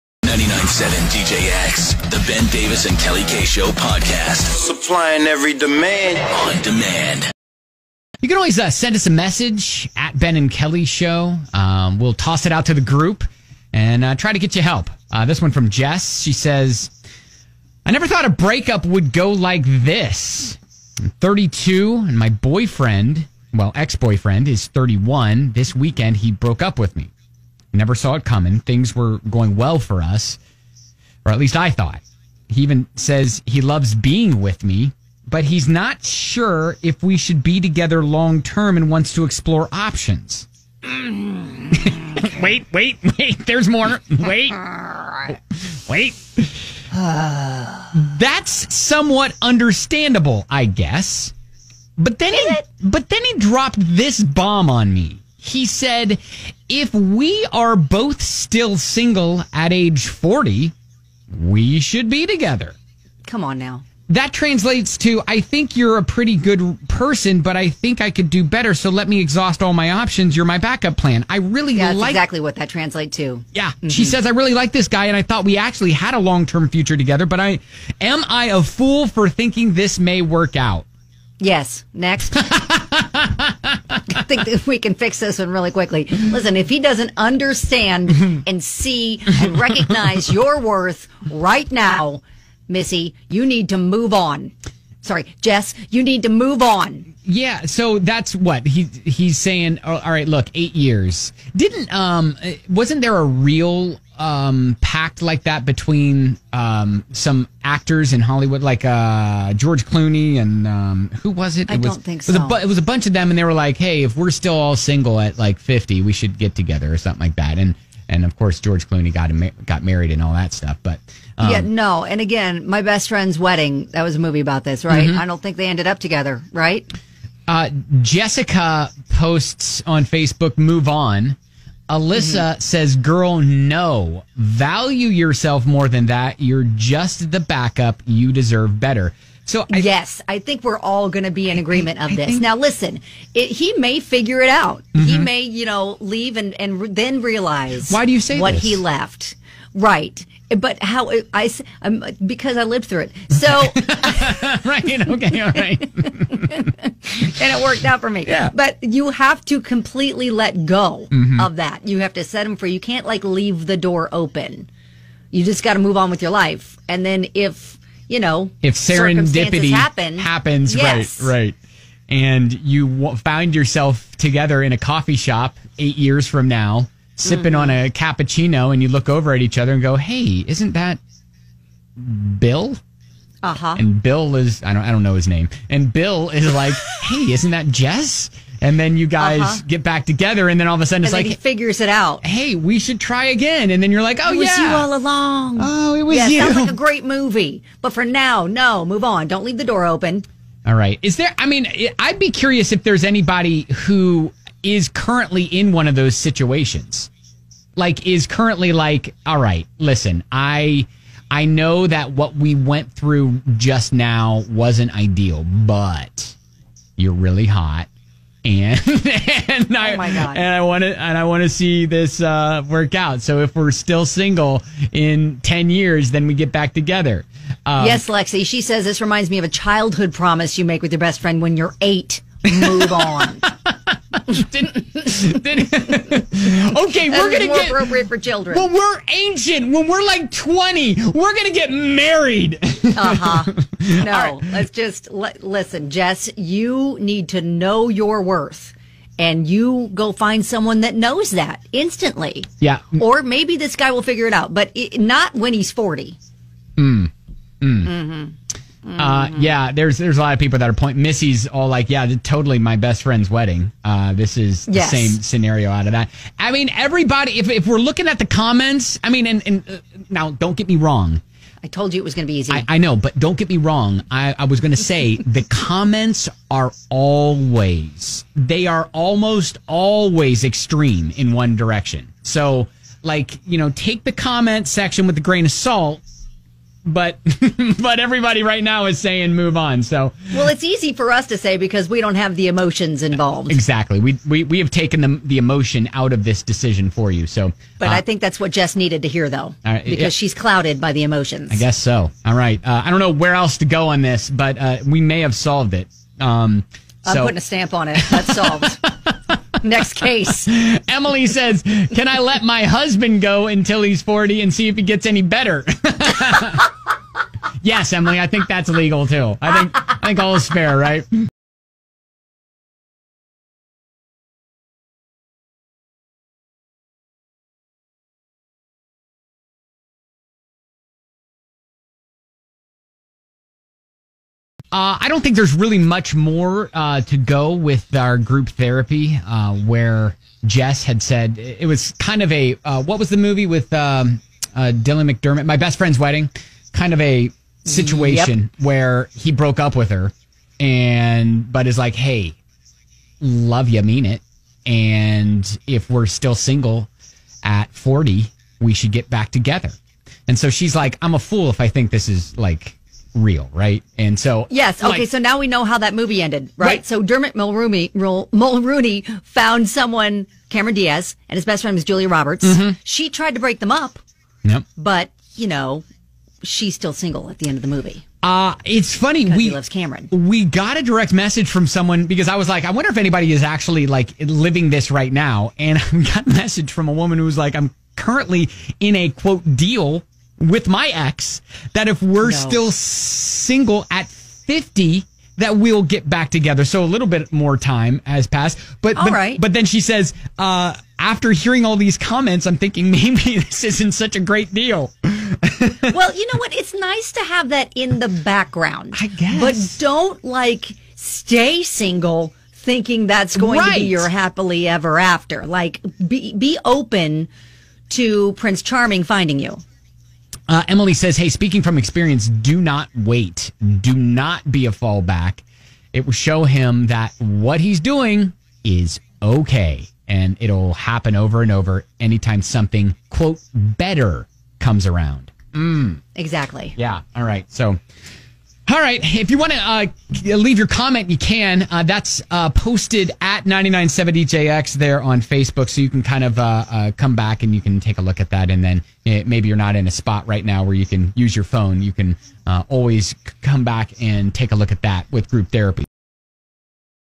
Group Therapy: His Backup Plan?
She was broken up with, but in a weird twist, he tells her he loves being with her and if they're both still single when they turn 40, they should get together again. Is she a "fool for thinking this may work out?"